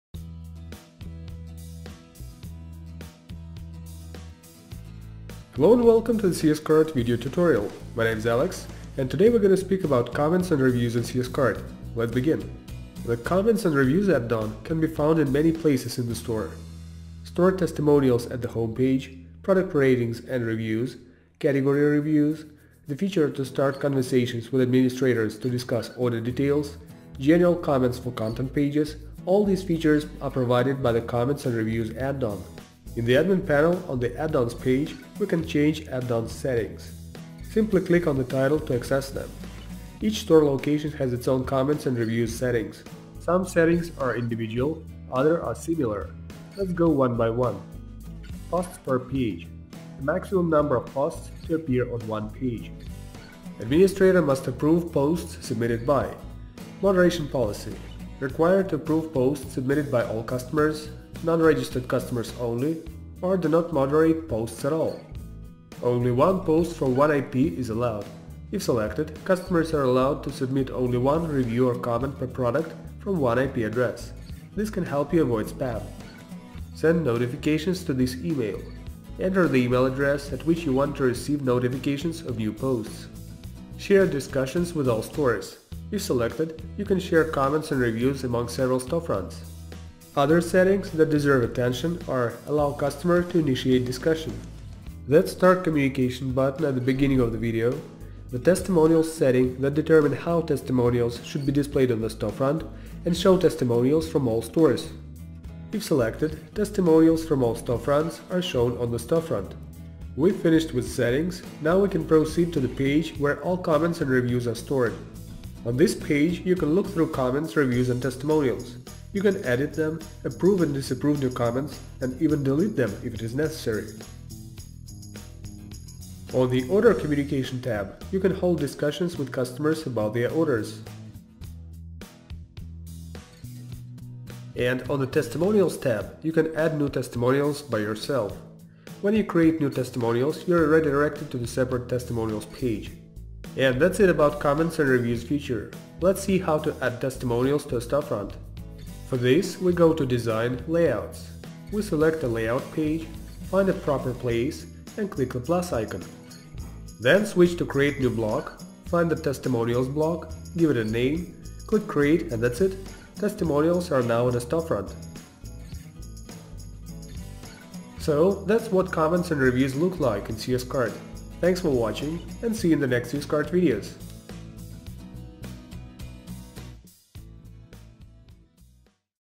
9.1. CS-Cart Software: Marketing — Comments & Reviews
The CS-Cart comments and reviews add-on once installed can be found in many places of the online store. Store testimonials on the homepage, product ratings and reviews, category reviews if enabled, the feature to start conversations with administrators to discuss order details, and general comments for content pages — all these provided by the Comments and Reviews add-on. Watch this video to learn more about comments and reviews in the CS-Cart ecommerce application.